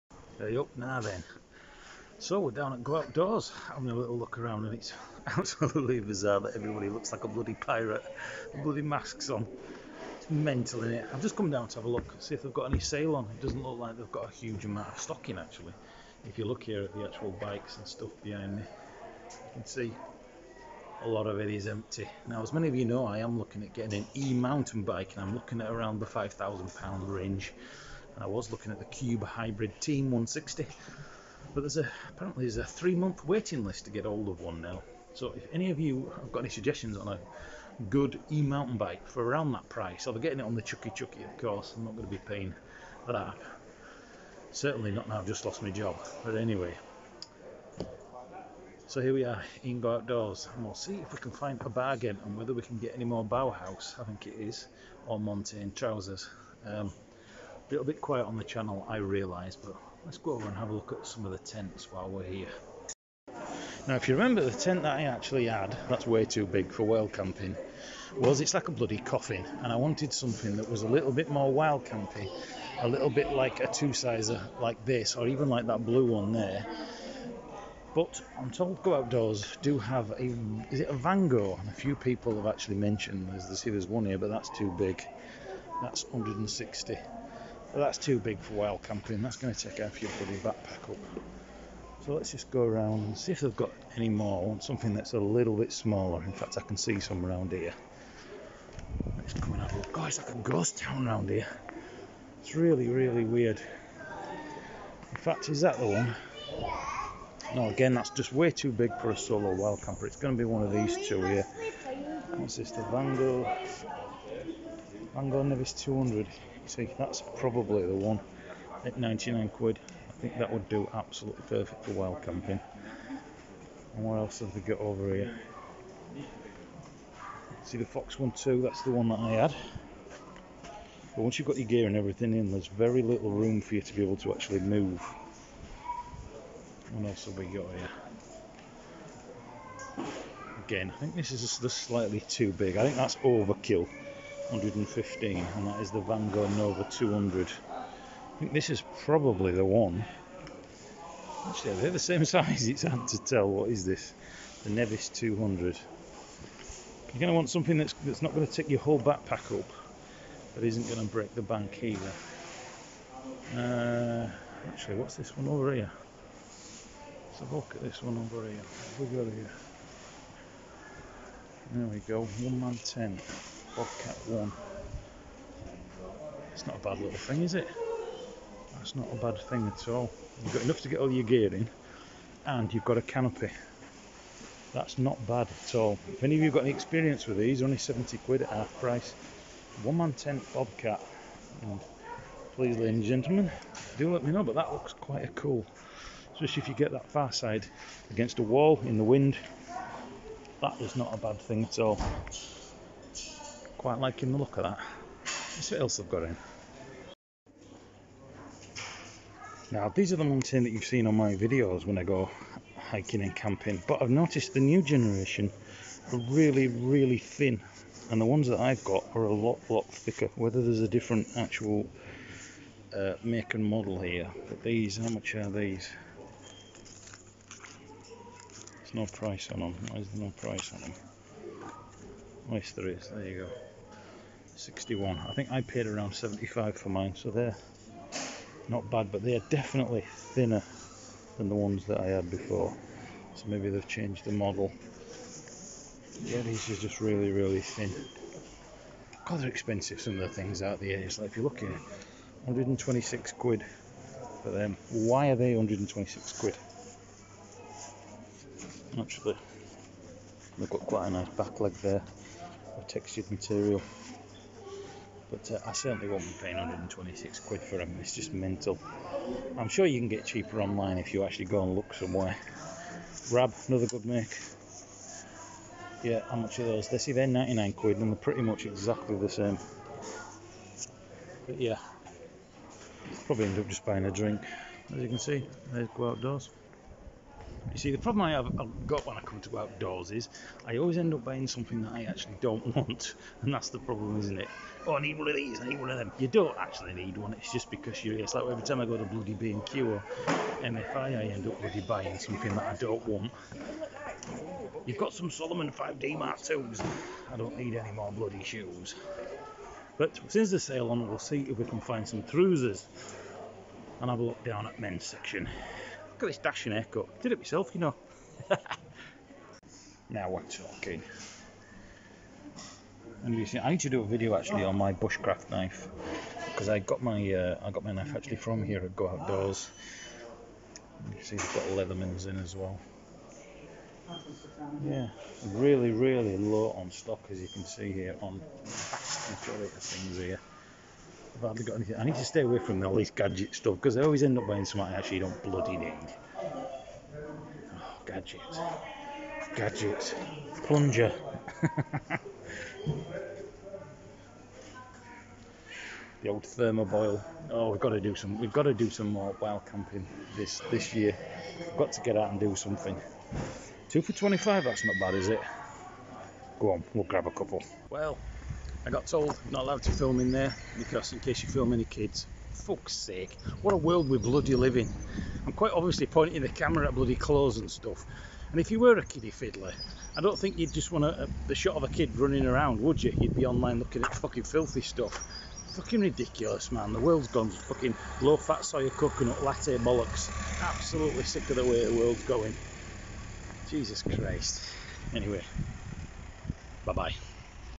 🌲 GO OUTDOORS TENTS 2020 - CANT FILM HERE!
Decided to have a little look around Go Out doors, not been for a while after my accident and having no car not been able to do much. Lets have a quick look around.

Got a location you want me to visit?
Any suggestions on videos?